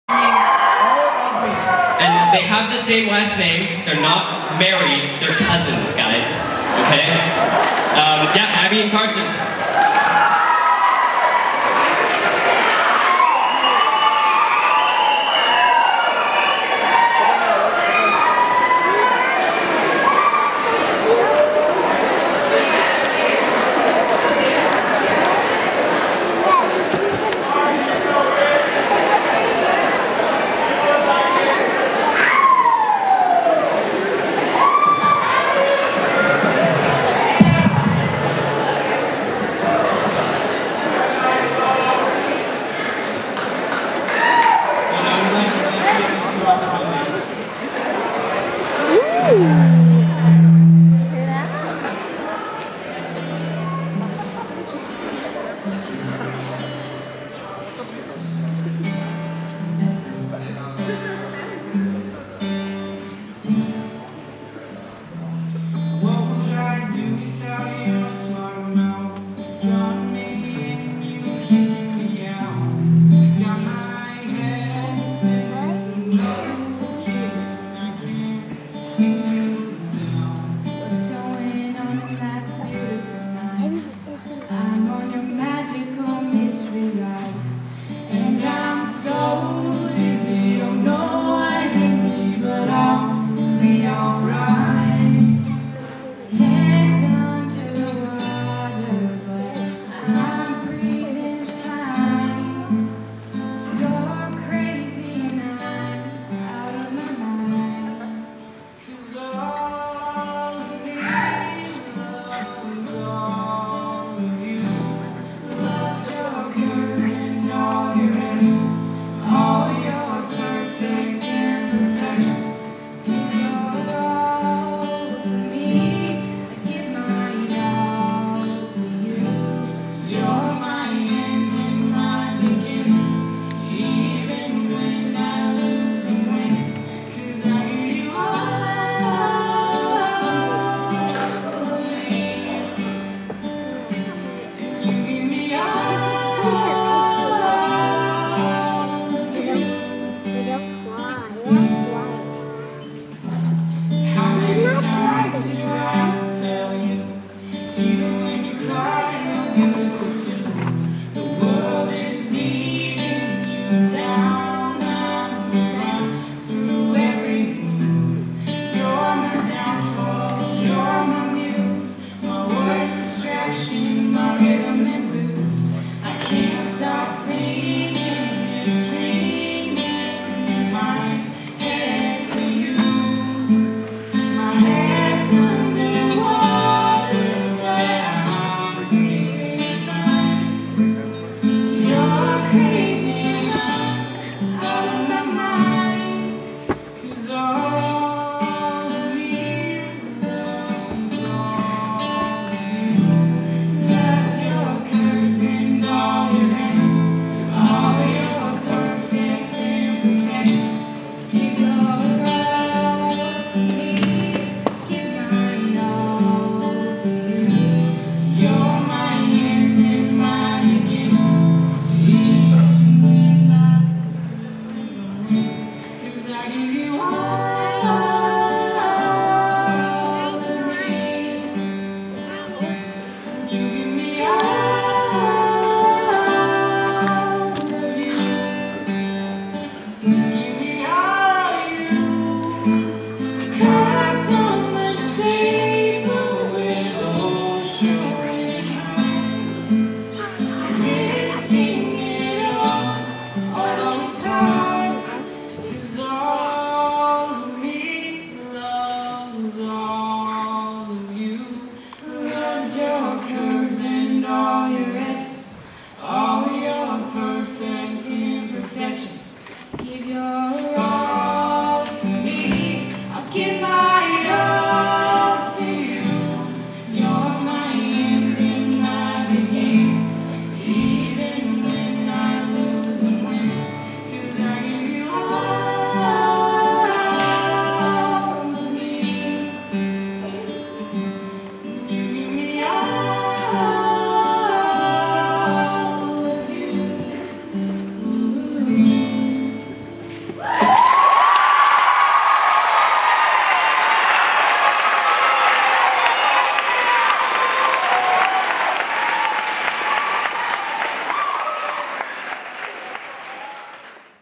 Abby & Karsten
Abby & Karsten at the prom assembly.